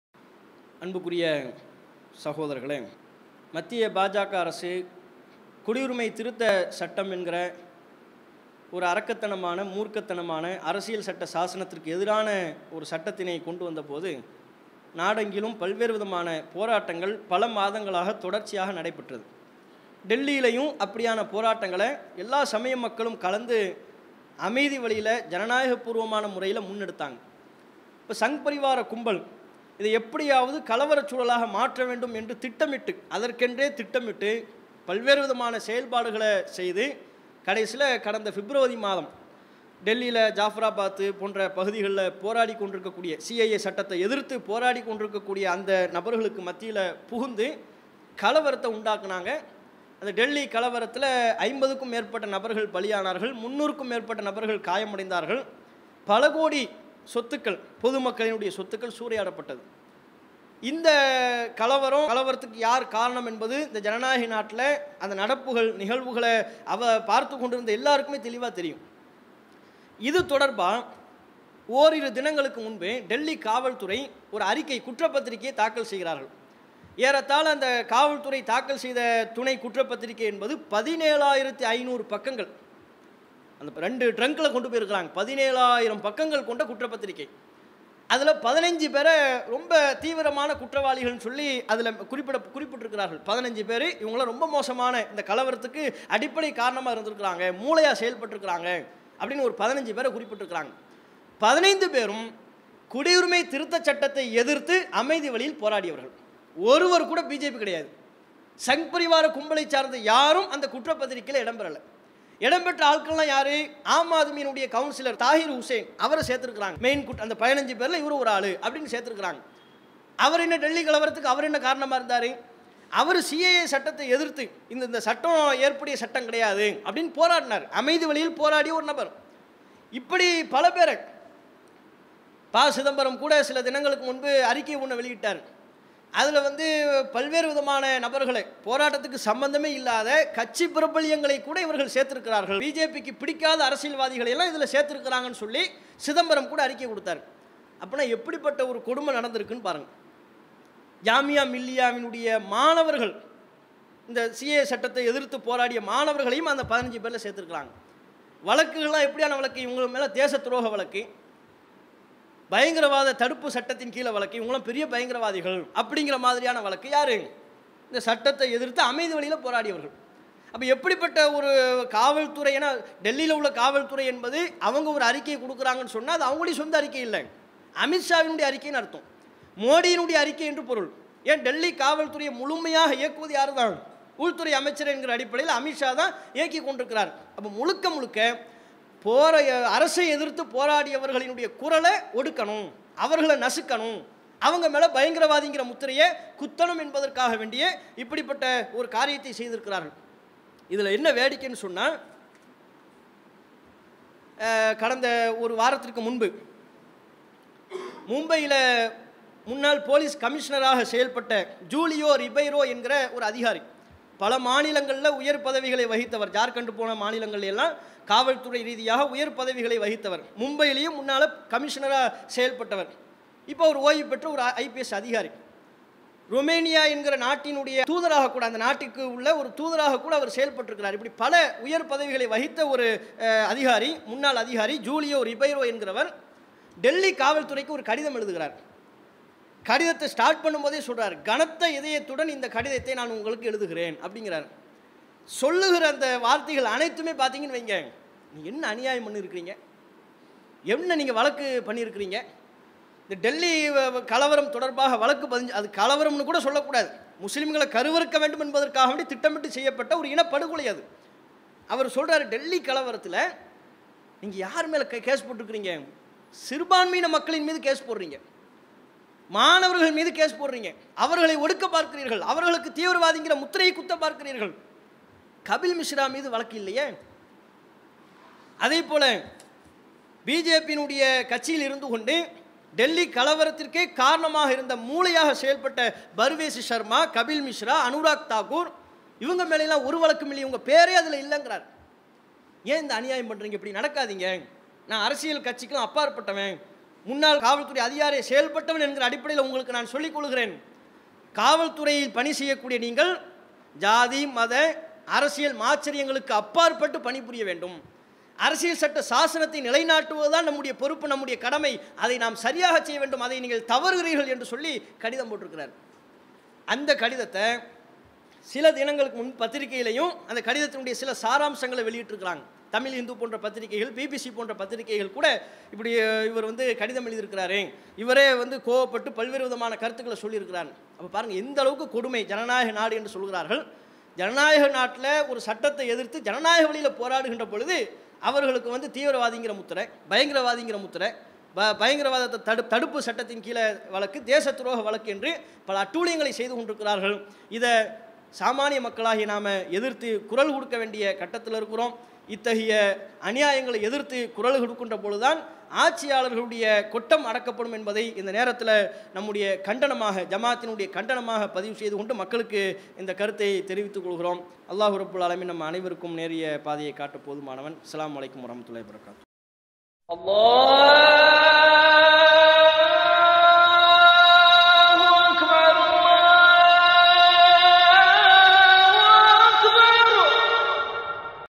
டெல்லி கலவரம் : அப்பாவிகளை பழிவாங்கிய டெல்லி காவல்துறை.!
வெள்ளி மேடை TNTJ தலைமையக‌ ஜுமுஆ இரண்டாம் உரை..!
உரை : சகோ. ஆர். அப்துல் கரீம் எம்.ஐ.எஸ்.ஸி - நாள் : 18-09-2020

இடம் : TNTJ மாநிலத் தலைமையகம் மண்ணடி வடசென்னை மாவட்டம்
தலைப்பு : டெல்லி கலவரம் - அப்பாவிகளை பழிவாங்கிய டெல்லி காவல்துறை.!